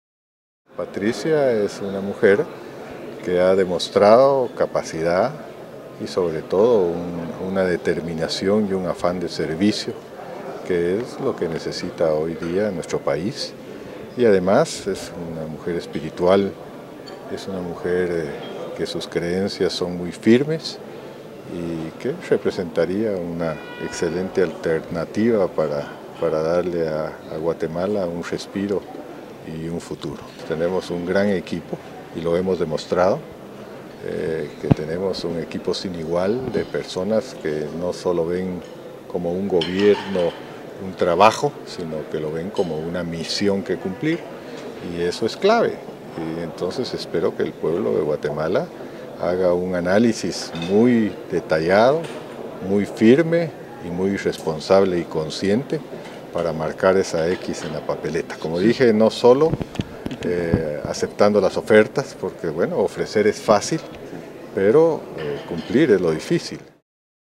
Patricia de Arzú para servir
Nuestro Alcalde, Alvaro Arzú comenta sobre la participación, dentro de la política de su esposa, Patricia de Arzú, quien tiene la capacidad y determinación por el servicio.